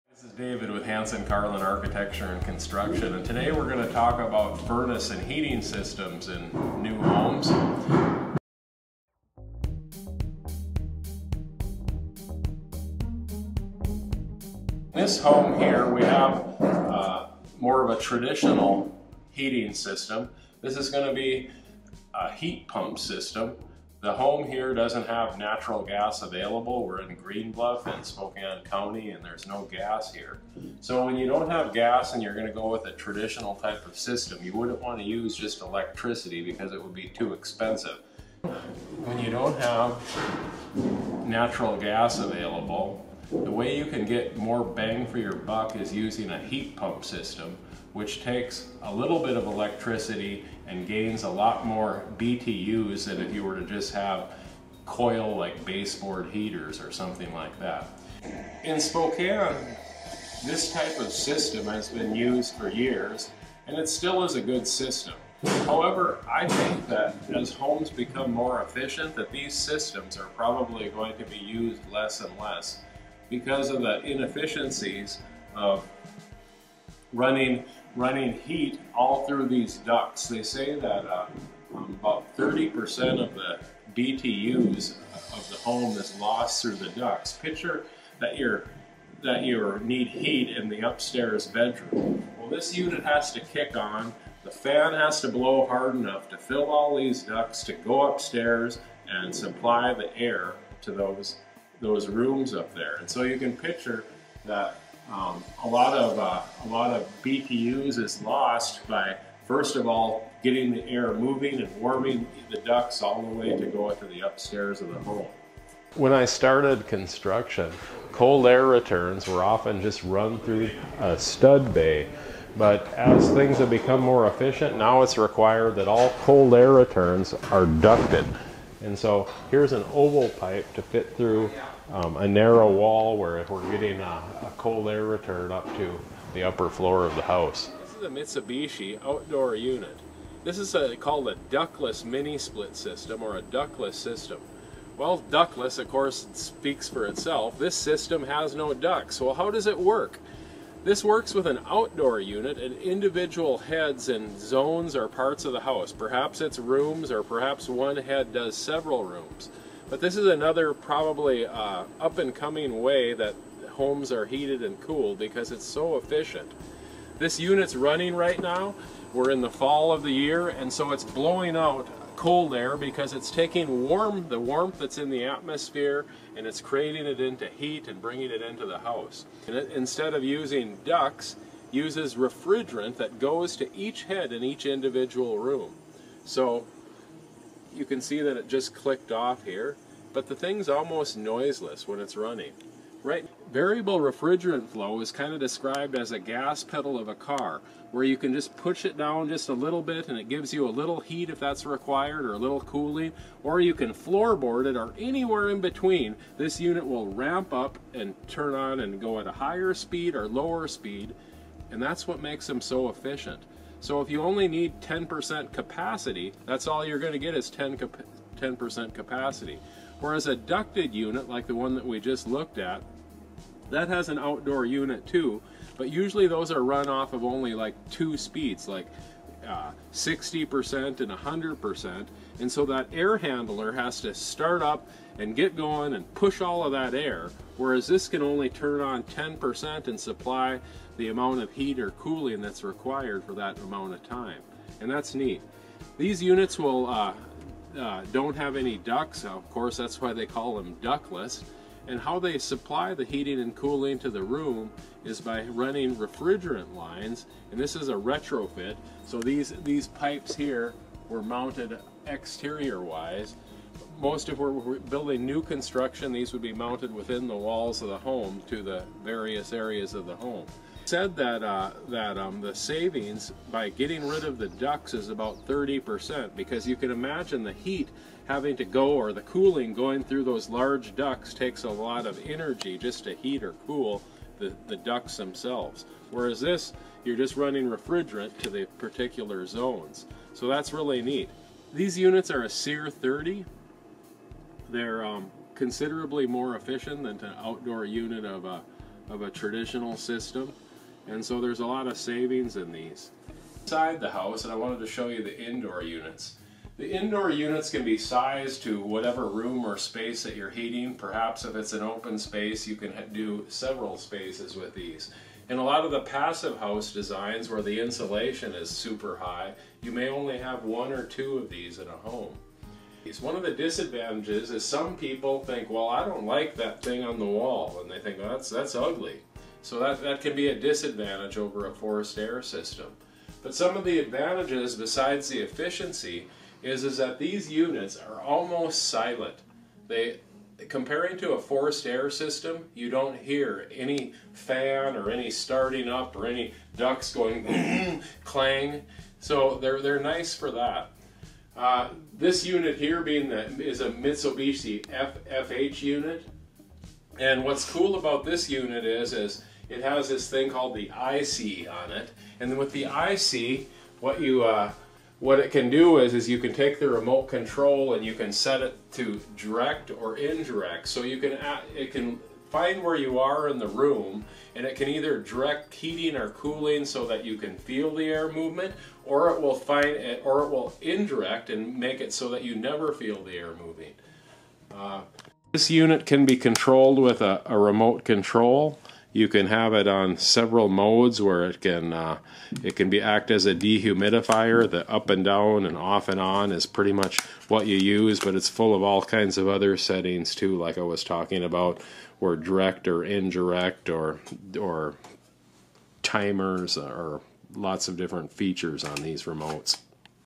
Benefits of Heat Pump and Mini Splits
** We are aware that the seer is 18 not 30, this was just misspoken at time of video**

Today there are many more heating options for your home than in days past. Such as the traditional heating systems which use a heat pump system or there are newer systems such as the Mitsubishi Mini-Split system. Some systems are more efficient than others.

It seems that more recently in the U.S. these min splits systems have become more popular and widespread in installation and use. The first ductless mini split system was invented in Japan in 1959. Mitsubishi is still to this day one of the top mini split producers. After much trial-and-error, Mitsubishi came up with the ductless system. These first systems were extremely large, clunky and manufactured close together; they were also very loud.

These systems became very popular in recent years in Europe and the Middle East but took much longer (decades) for the U.S. to start appreciating and using.

At Hanson Carlen, we have found that these mini split units are more efficient than your typical HVAC system and can help with energy bill savings. They make less noise, and they are less bulky, as the manufacturing company has worked on and found ways to slimline the body of the units.